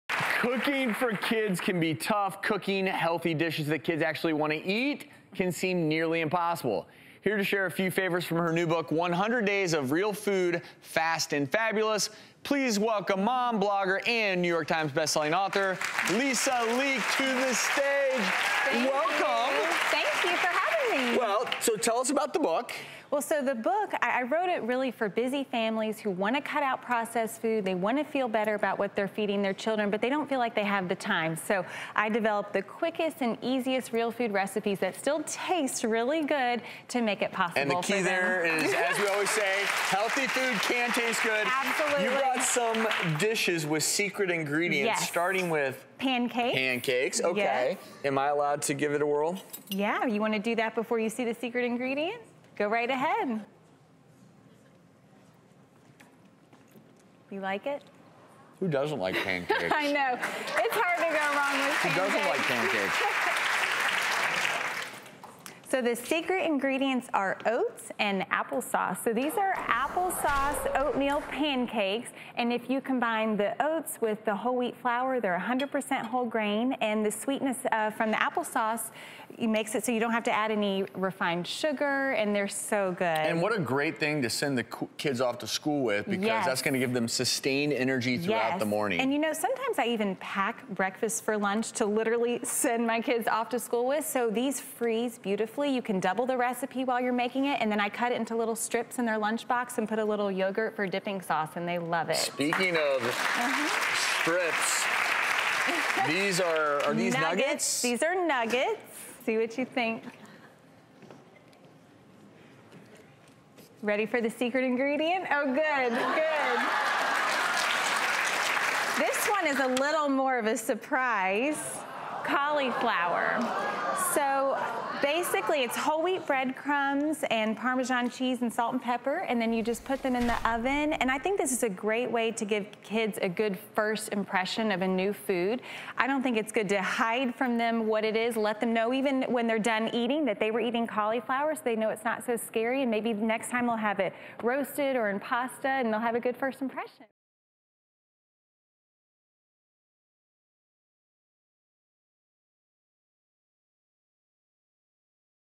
Healthy Food Swaps Your Kids Will Love!
Mom blogger and author Lisa Leake shares how to make some of your kid's favorite foods healthier.